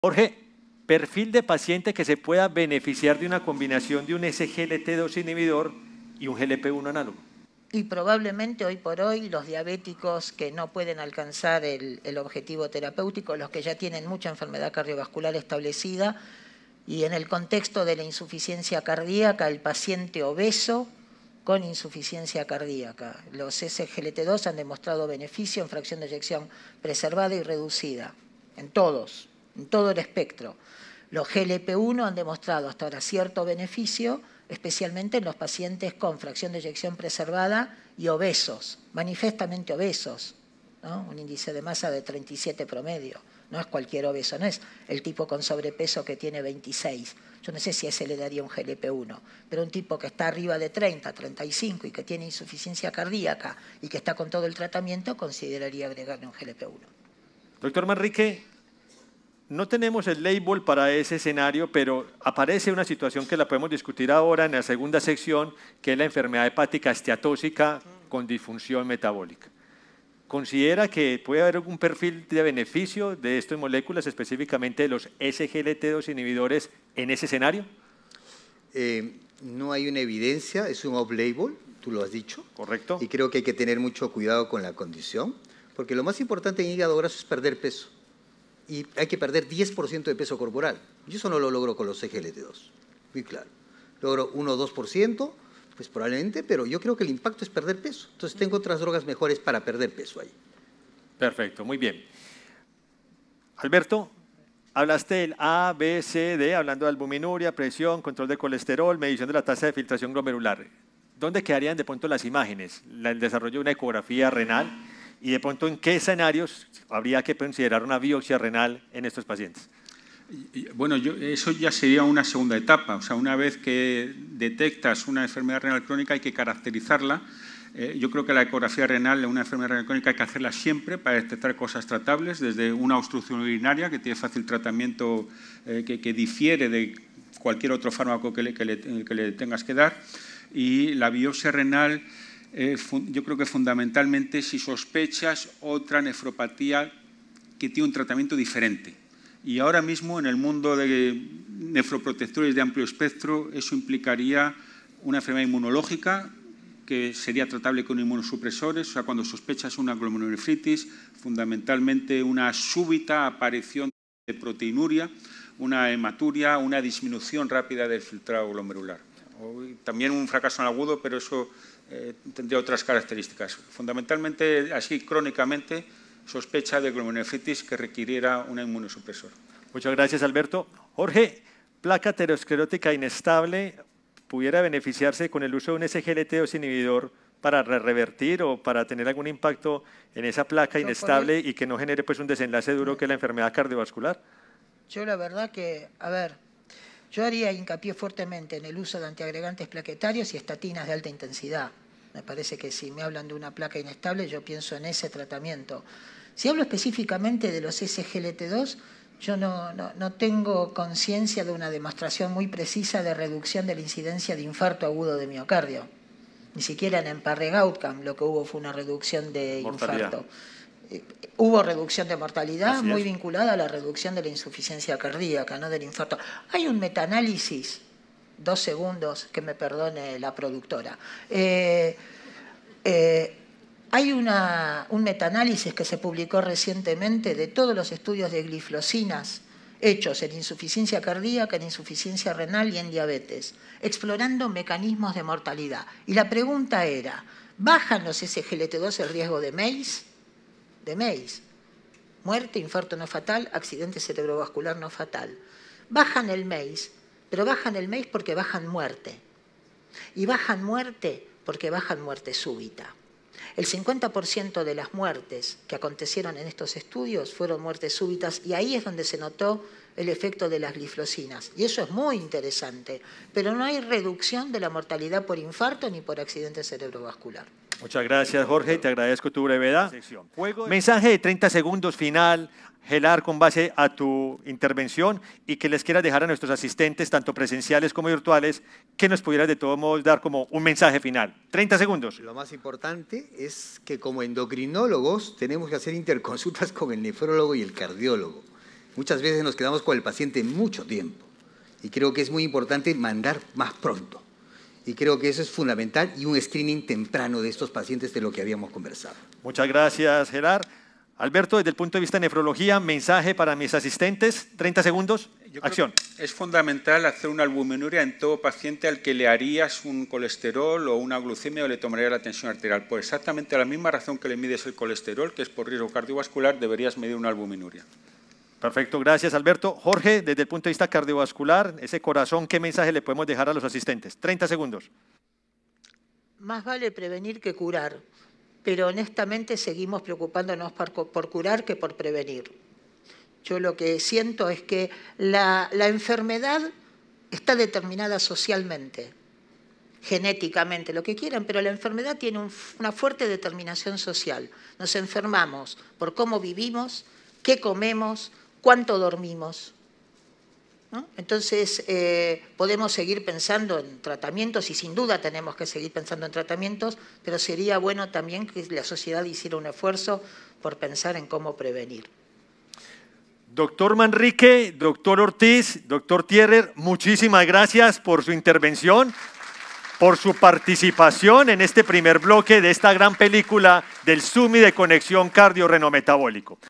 2 ¿Cómo usar los inhibidores de SGLT2 en riesgo cardiorrenometabólico?
Participan:
Dr. Alberto Ortíz, Médico Nefrólogo (España)
Dr. Helard Manrique, Médico Endocrinólogo (Perú)
Dr. Jorge Thrierer, Médico cardiólogo (Argentina)
Dr. José Luis Torres, Médico Endocrinólogo (Colombia)

Lugar: Lima, Perú.

00:00 Combinación de inhibidor SGLT2 y análogo de GLP-1
01:10 Inhibidores de SGLT2 en NASH
01:50 Ecografía renal y biopsia renal en IRC
03:07 ¿iSGLT2 en placa inestable coronaria?
04:40 Mensajes finales